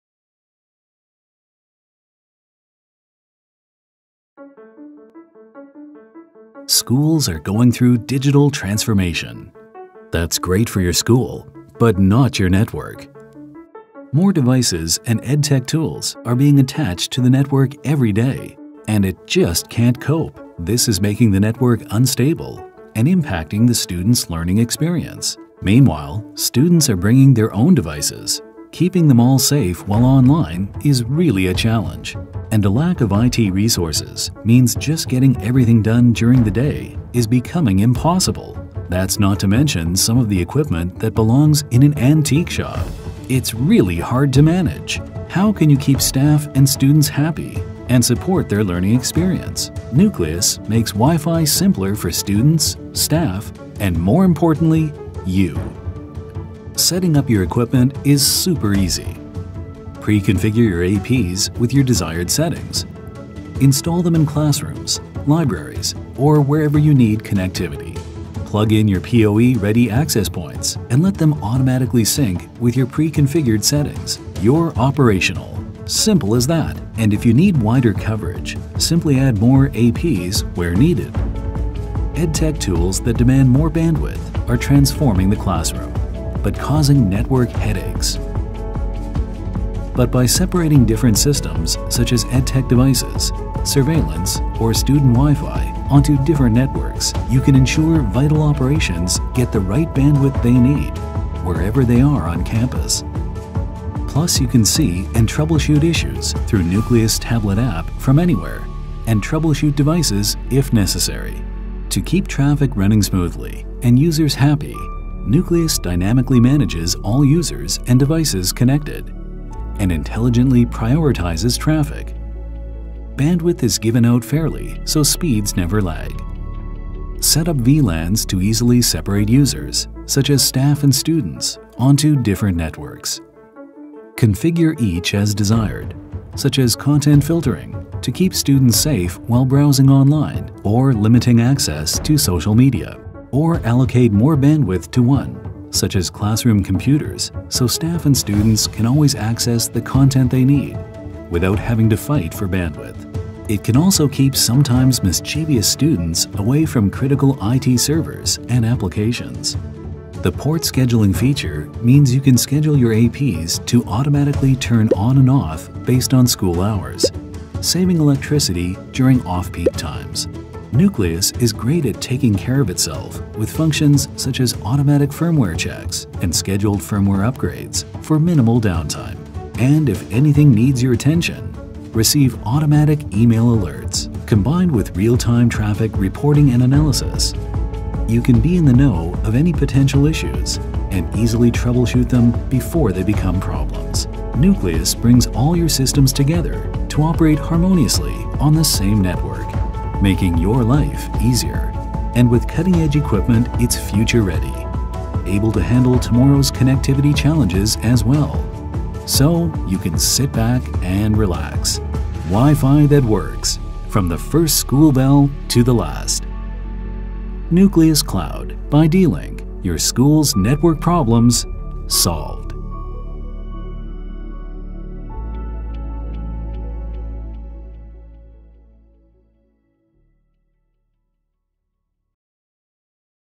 Instalar Redes WiFi Colegios Universidades gestion red Cloud Managed D Link Nuclias
Solución integral para mejorar redes WiFI en colegios, universidades y sector educación. Puntos de Acceso de alto rendimiento y gestión de red con segmentación por aulas, profesores, etc. Control total del tráfico y estado de la red. Los puntos de acceso y switches PoE pueden enviarse a los centros educativos ya preconfigurados para que se aprovisionen en la configuración de red establecida simplemente con enchufarlos y conectarlos a Internet. Capacidad de gestión remota Multisede para proveedores de servicios o para ofrecer la gestón de red como servicio añadido y evitar el envio de técnicos.